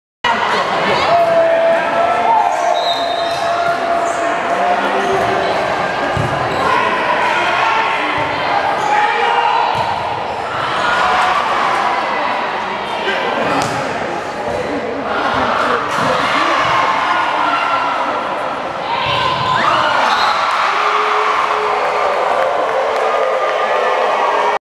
Lauren Sutrin Makes The Incredible Block At The Net
San Pedro vs. Mary Star Girls Volleyball Highlights (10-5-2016)